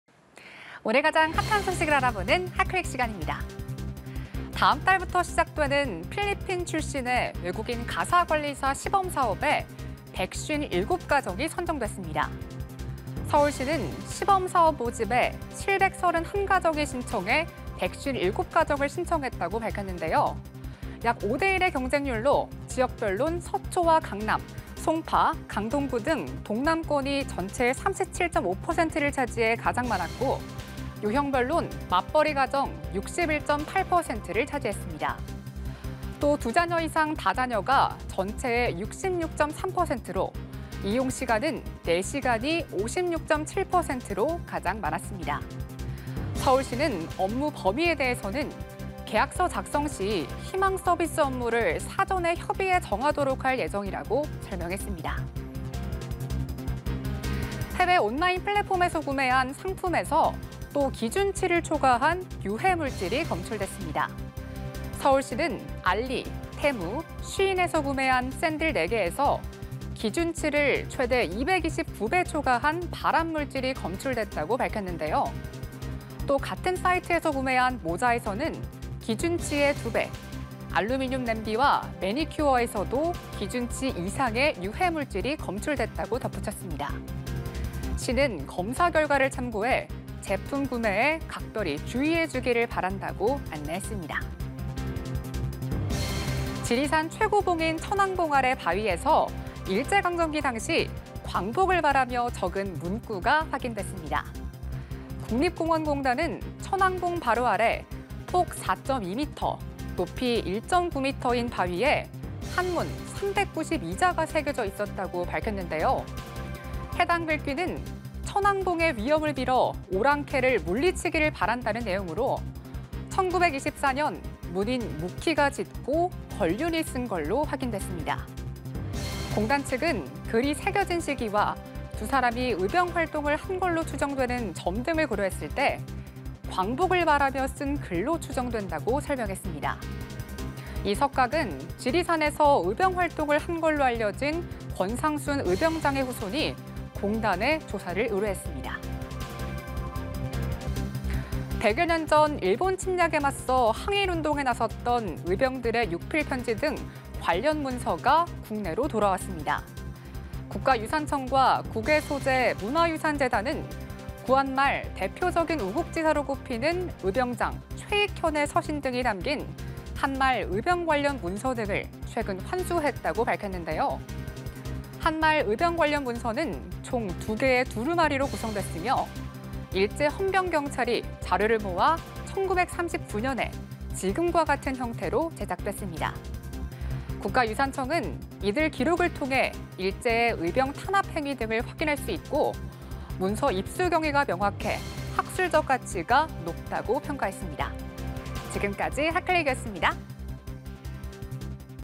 [핫클릭] 필리핀 가사관리사 시범사업 157가정 선정…경쟁률 5대 1 外 / 연합뉴스TV (YonhapnewsTV)
[핫클릭] 필리핀 가사관리사 시범사업 157가정 선정…경쟁률 5대 1 外

오늘 가장 핫한 소식을 알아보는 [핫클릭]시간입니다.

▶ 필리핀 가사관리사 시범사업 157가정 선정…경쟁률 5대 1

다음 달부터 시작되는 필리핀 출신의 외국인 가사관리사 시범사업에 157가정이 선정됐습니다.

서울시는 시범사업 모집에 731가정이 신청해 157가정을 선정했다고 밝혔습니다.

약 5대 1의 경쟁률로, 지역별로는 서초와 강남, 송파, 강동구 등 동남권이 전체의 37.5%를 차지해 가장 많았고, 유형별로는 맞벌이 가정 61.8%를 차지했습니다.

또 2자녀 이상 다자녀가 전체의 66.3%로, 이용시간은 4시간이 56.7%로 가장 많았습니다.

서울시는 업무 범위에 대해서는 계약서 작성 시 희망 서비스 업무를 사전에 협의해 정하도록 할 예정이라고 설명했습니다.

▶ 알·테·쉬 샌들서 기준치 229배 초과 발암물질 검출

해외온라인 플랫폼에서 구매한 상품에서 또 기준치를 초과한 유해물질이 검출됐습니다.

서울시는 알리·테무·쉬인에서 구매한 샌들 4개에서 기준치를 최대 229배 초과한 발암물질이 검출됐다고 밝혔습니다.

또 같은 사이트에서 구매한 모자에서는 기준치의 2배, 알루미늄 냄비와 매니큐어에서도 기준치 이상의 유해물질이 검출됐다고 덧붙였습니다.

시는 검사 결과를 참고해 제품 구매에 각별히 주의해 주기를 바란다고 안내했습니다.

▶ 지리산 천왕봉 아래 바위서 '광복 염원' 글귀 392자 확인

지리산 최고봉인 천왕봉 아래 바위에서 일제강점기 당시 광복을 바라며 적은 문구가 확인됐습니다.

국립공원공단은 천왕봉 바로 아래 폭 4.2m, 높이 1.9m인 바위에 한문 392자가 새겨져 있었다고 밝혔습니다.

해당 글귀는 '천왕봉의 위엄을 빌어 오랑캐를 물리치기를 바란다'는 내용으로, 1924년 문인 묵희가 짓고 권륜이 쓴 것으로 확인됐습니다.

공단 측은 글이 새겨진 시기와 두 사람이 의병 활동을 한 것으로 추정되는 점 등을 고려했을 때 광복을 바라며 쓴 글로 추정된다고 설명했습니다.

이 석각은 지리산에서 의병 활동을 한 것으로 알려진 권상순 의병장의 후손이 공단에 조사를 의뢰했습니다.

▶ 항일 의병장들의 편지 등 110년 만에 일본서 환수

100여 년 전 일본 침략에 맞서 항일 운동에 나섰던 의병들의 육필 편지 등 관련 문서가 국내로 돌아왔습니다.

국가유산청과 국외소재문화유산재단은 구한말 대표적인 우국지사로 꼽히는 의병장 최익현의 서신 등이 담긴 '한말 의병 관련 문서' 등을 최근 환수했다고 밝혔습니다.

'한말 의병 관련 문서'는 총 2개의 두루마리로 구성됐으며, 일제 헌병경찰이 자료를 모아 1939년에 지금과 같은 형태로 제작됐습니다.

국가유산청은 "이들 기록을 통해 일제의 의병 탄압행위 등을 확인할 수 있고, 문서 입수경위가 명확해 학술적 가치가 높다"고 평가했습니다.